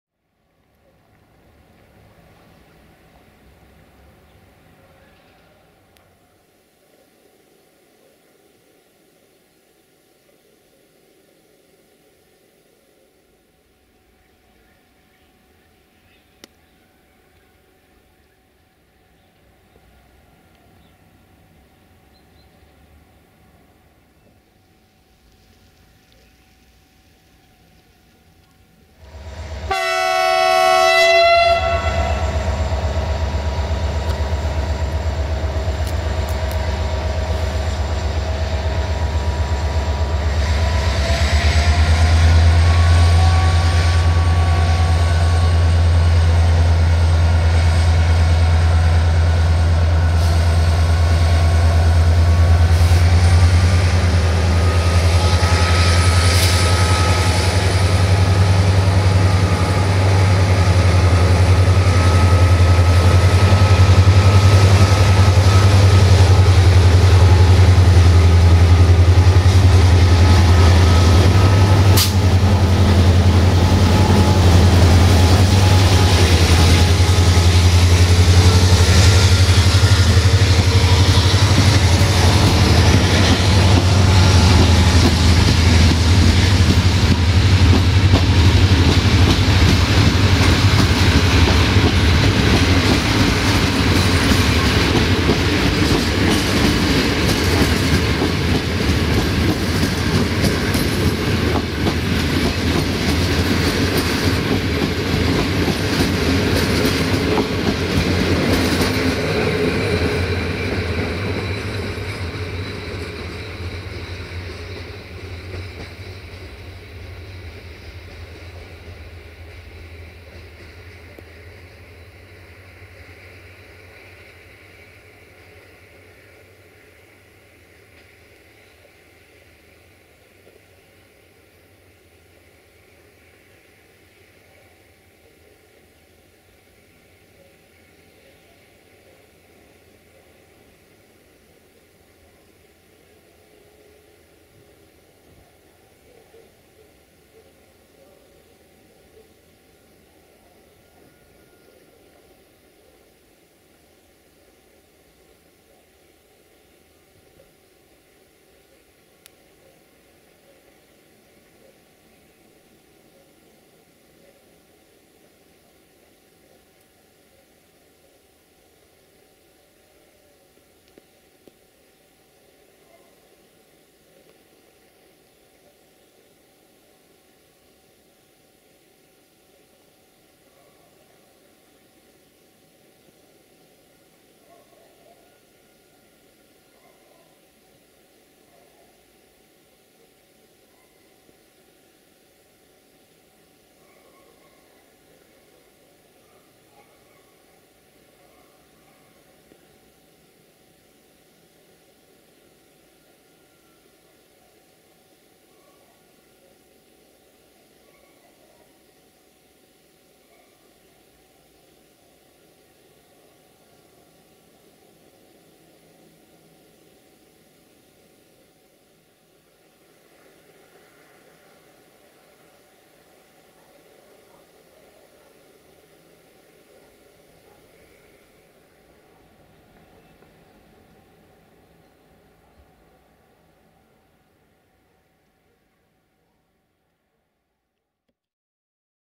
New Jimmy 64-1019-0 & IR1741 București Nord-Satu Mare in Gara Valea lui Mihai Station - 21 Dec 2019
New Jimmy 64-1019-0 & IR1741 București Nord-Satu Mare in Gara Valea lui Mihai Station - 21 December 2019

1:IR1741 - București Nord-Ploiești Vest-Sinaia-Predeal-Brașov-Sighișoara-Teiuș-Războieni-Câmpia Turzii-Cluj Napoca-Aghireș-Huegin-Bratca-Aleșd-Oradea-Săcuieni Bihor-Valea lui Mihai-Carei-Satu Mare - Jimmy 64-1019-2 RO-SNTFC

21 December 2019